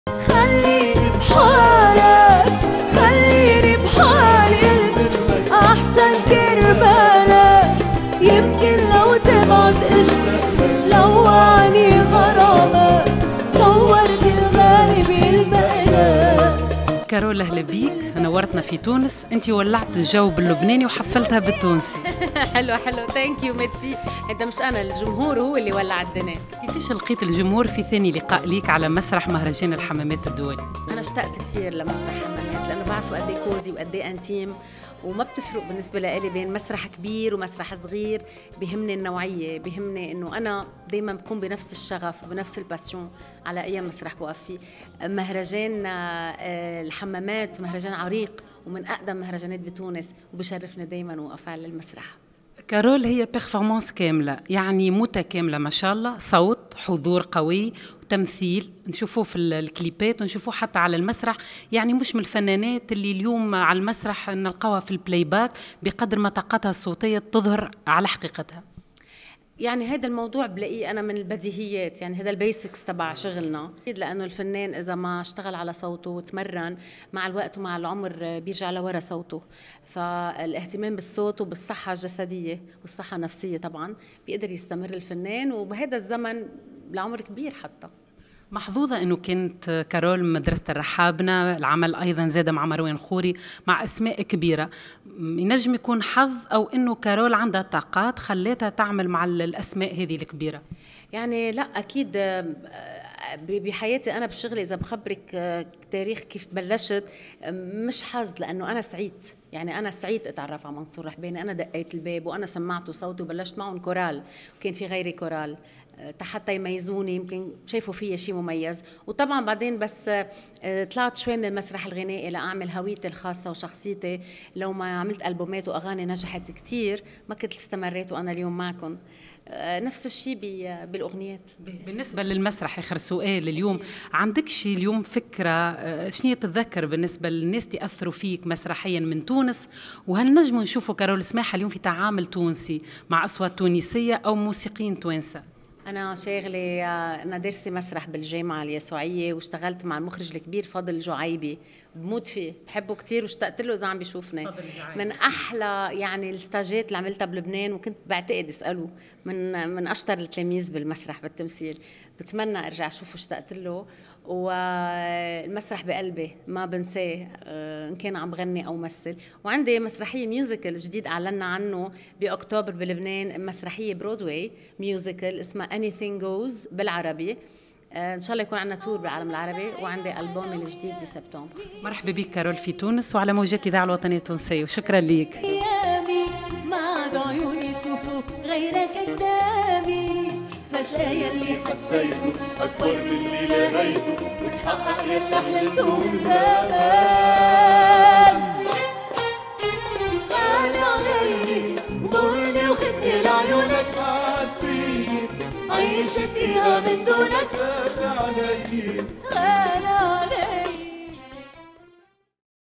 كارول سماحة للإذاعة الوطنية: يشرفني الوقوف على مسرح الحمامات وأنا تلميذة الجعايبي
Festival International de Hammamet